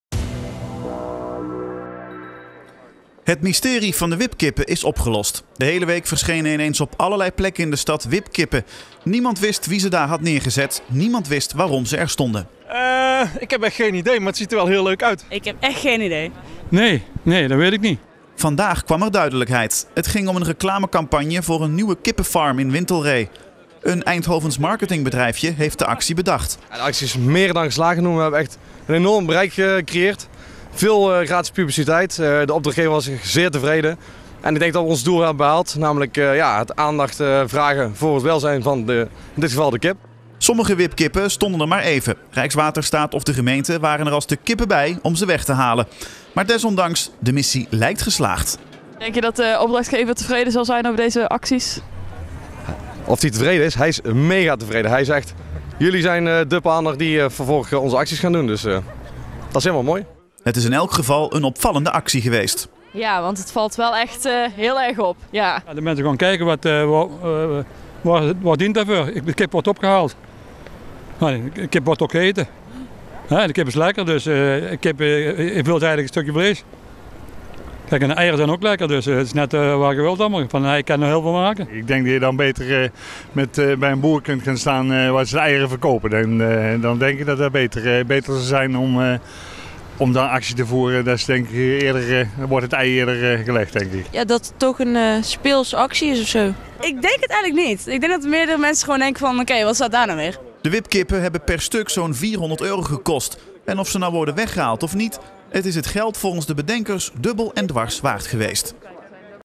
Mysterie wipkip opgelost
De laatste tijd doken op verschillende plaatsen in de stad uit het niets wipkippen op. Waar ze vandaan kwamen wist niemand. Dat mysterie is nu opgelost.